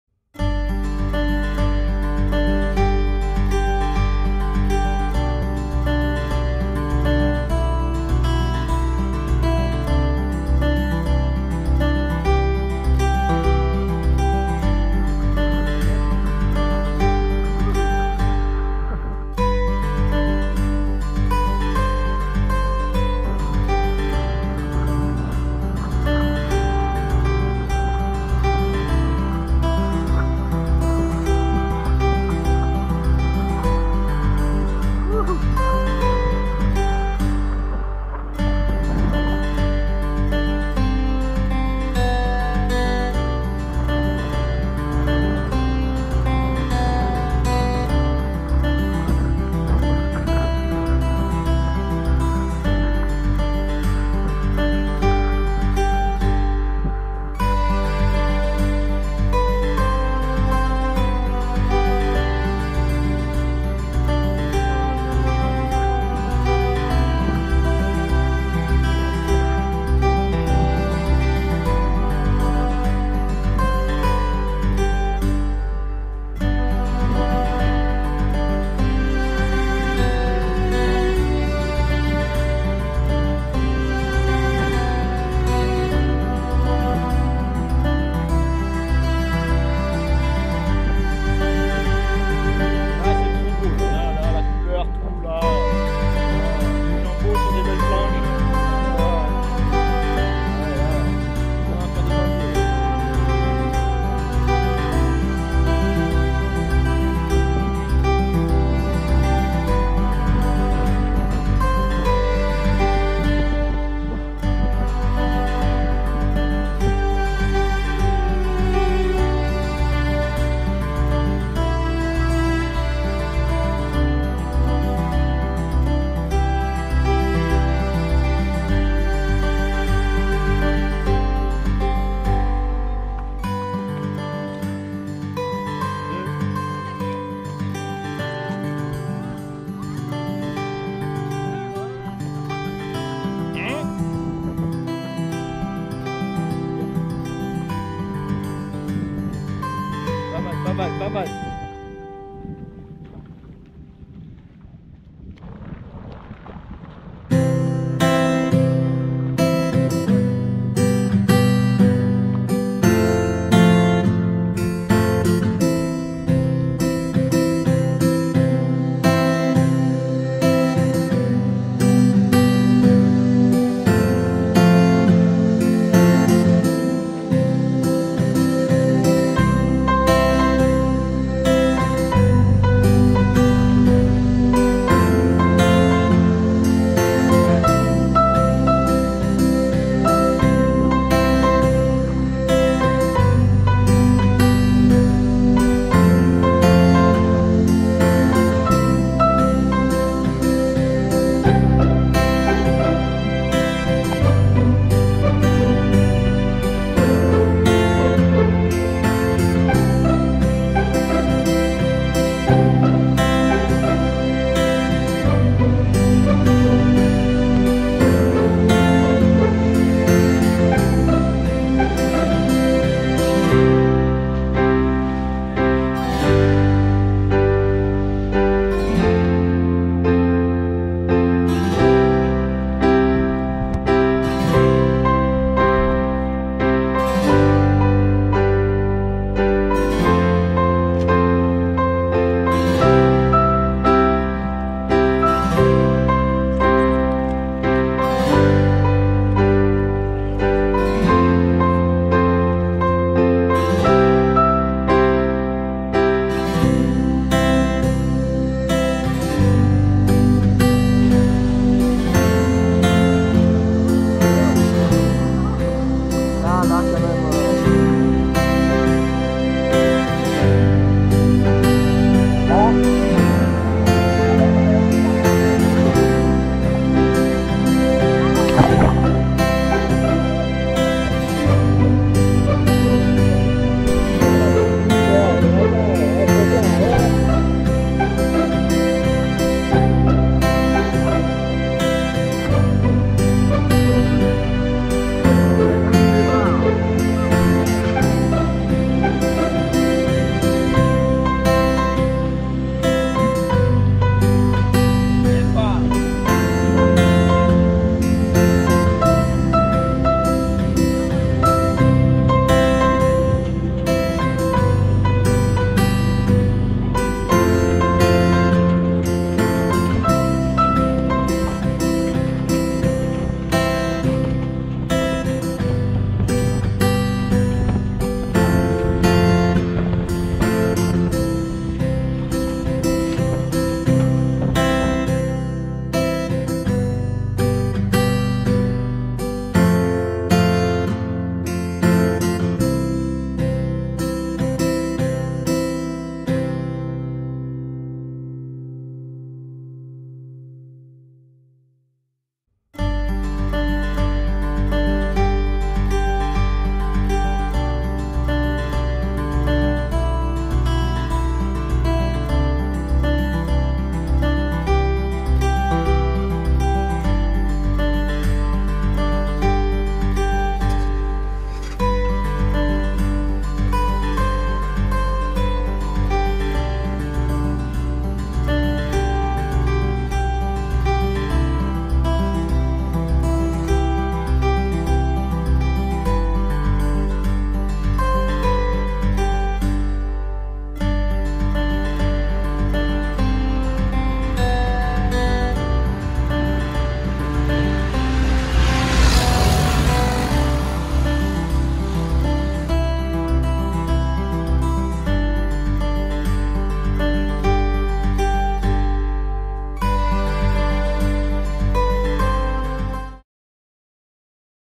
Sup st Barth
Balade en paddle de Gouverneur à Public, à St Barth